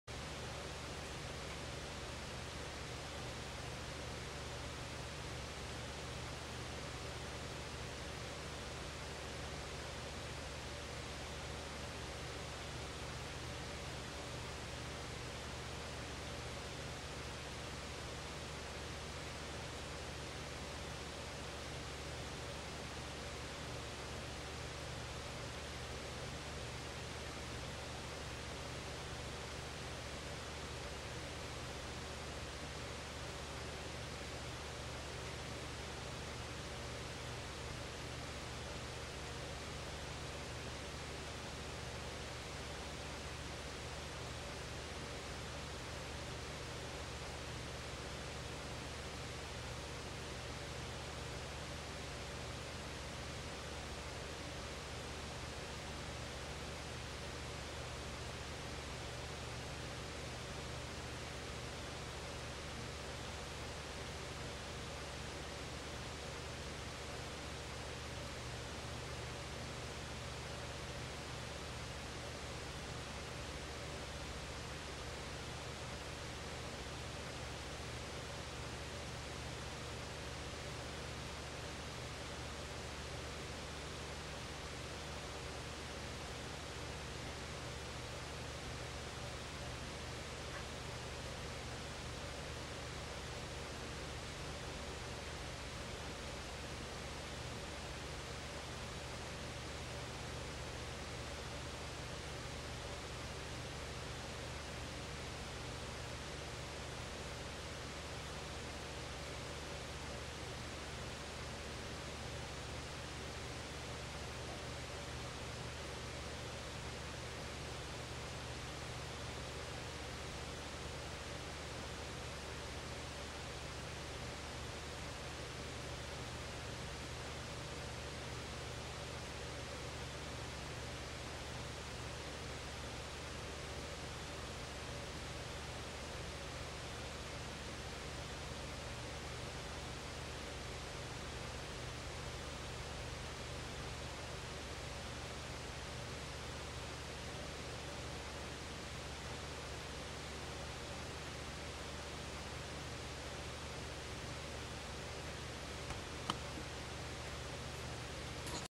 American Tree Sparrow Up Close
The bluejay showing up was inevitable with that peanut right there.